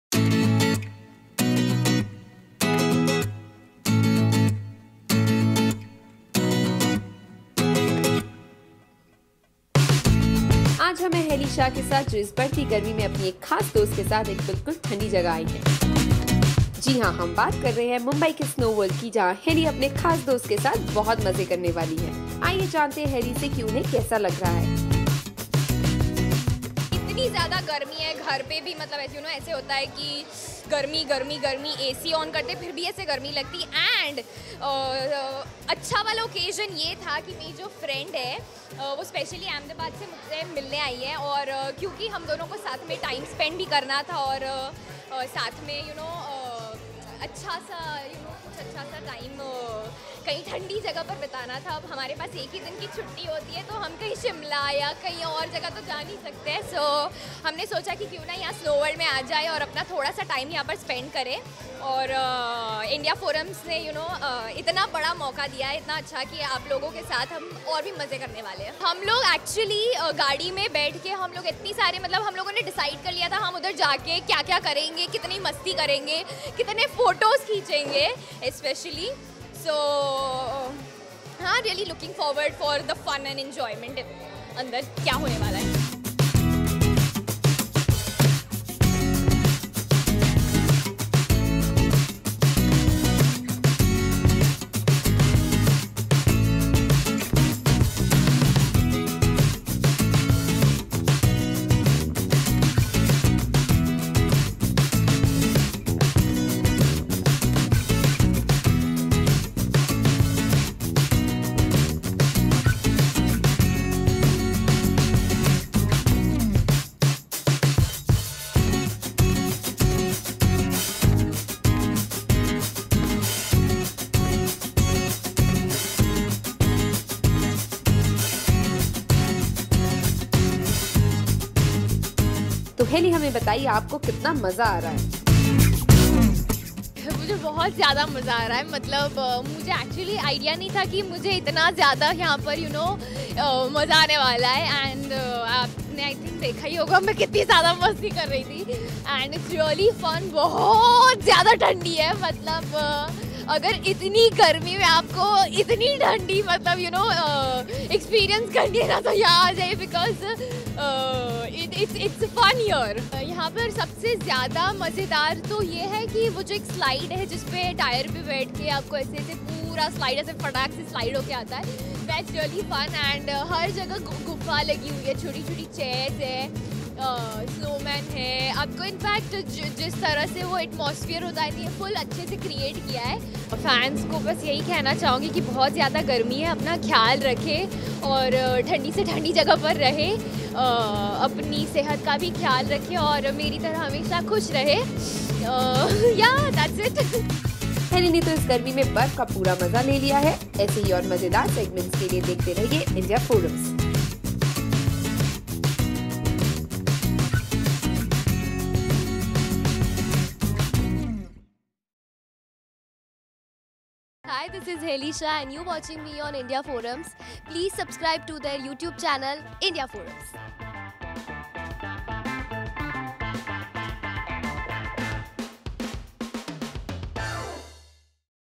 Helly Shah has fun in the snow with a special friend!
Swaragini's Swara aka Helly Shah has found the perfect place to beat the heat. Check out the video as we take you through Helly's fun time at snow world with a special someone!